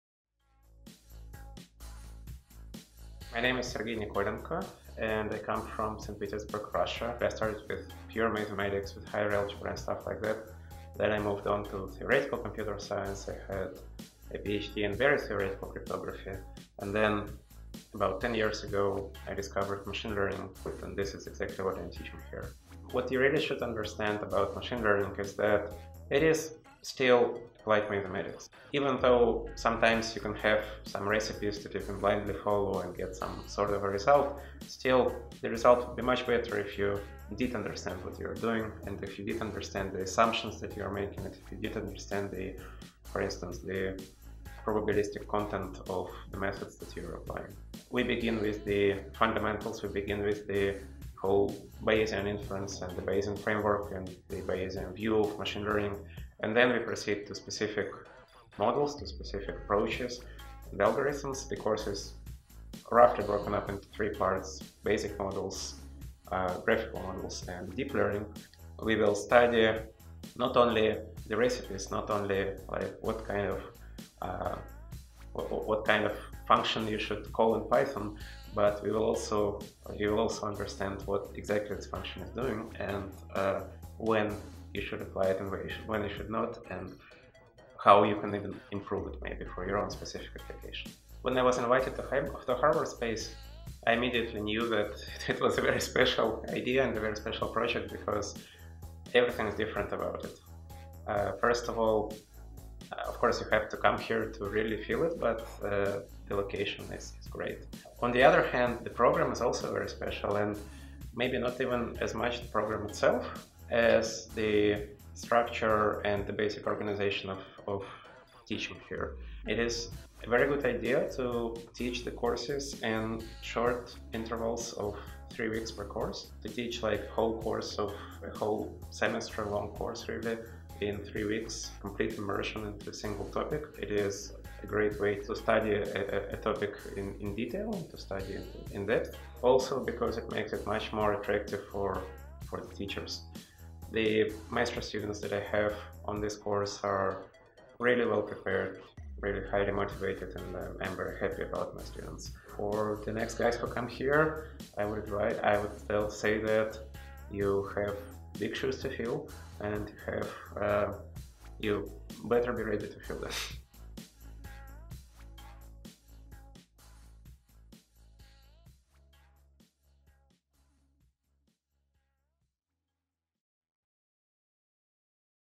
Sergey Nikolenko x Data Science Program
Sergey Nikolenko is a Senior Researcher, Steklov Institute of Mathematics at St. Petersburg Russia,  who has been studying in the field of Machine Learning for the past 10 years. He has provided research for many institutions as well as being an advisor of teams for many research projects. A friendly inspiring individual who can turn any complex information into a fun and simple understanding. This experience is extremely essential to the computer/data science students here at Harbour University because it can help understand and achieve advanced learning curves of research and analytics by learning to understand the complex material of machine learning while keeping it simple and creative. In this Data Science course Sergey is teaching how to understand to use Machine Learning effectively.